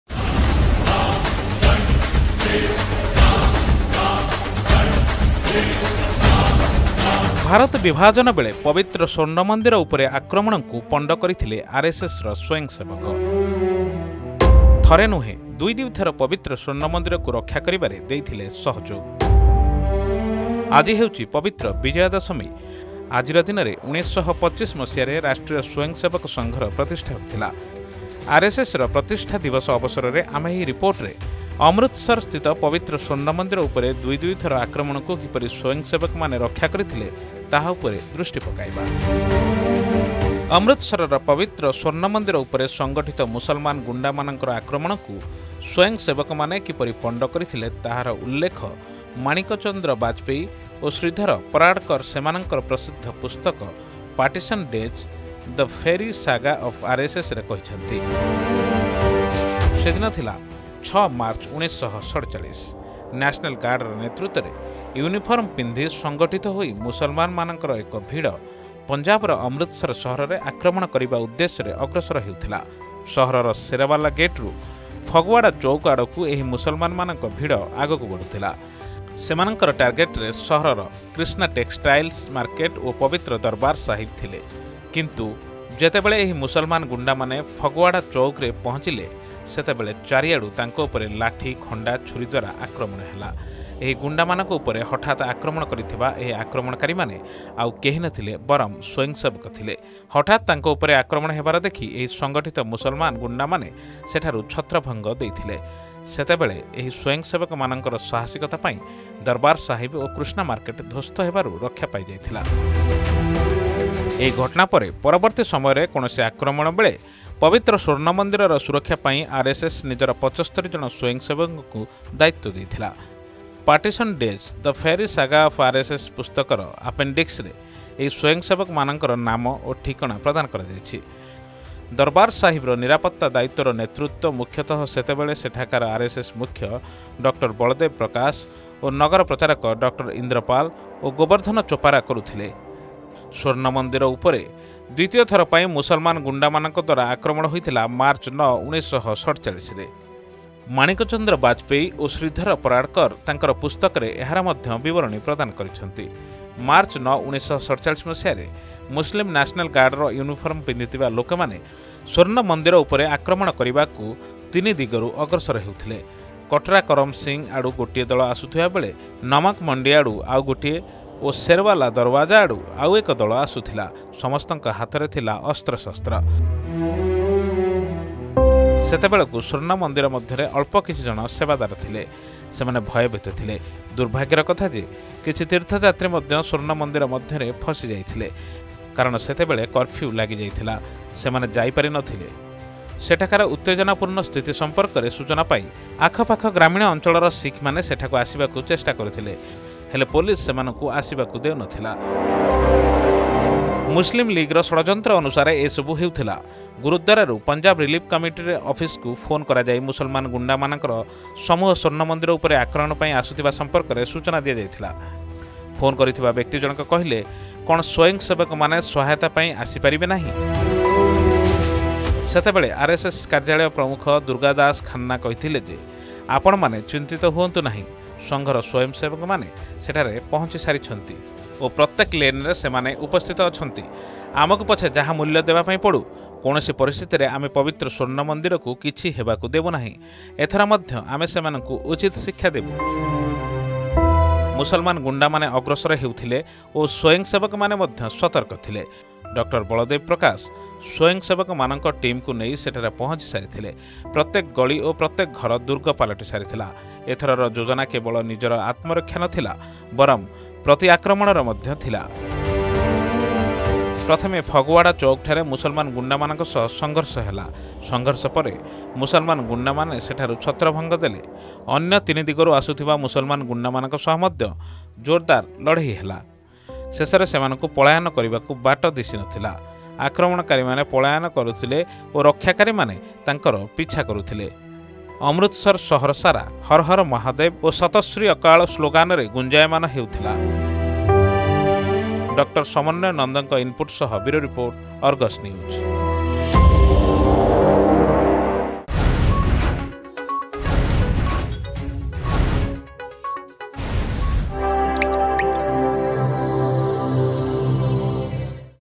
ରାଷ୍ଟ୍ରୀୟ ସ୍ବୟଂସେବକ ସଂଘର ବୀରତ୍ବ ଗାଥା
ଭାରତ ବିଭାଜନ ବେଳେ ପବିତ୍ର ସ୍ବର୍ଣ ମନ୍ଦିର ଉପରେ ଆକ୍ରମଣକୁ ପଣ୍ଡ କରିଥିଲେ ଆରଏସଏସର ସ୍ୱୟଂସେବକ I ଥରେ ନୁହେଁ,  ଦୁଇ - ଦୁଇ ଥର ପବିତ୍ର ସ୍ୱର୍ଣମନ୍ଦିରକୁ ରକ୍ଷା କରିବାରେ ଦେଇଥିଲେ ସହଯୋଗ I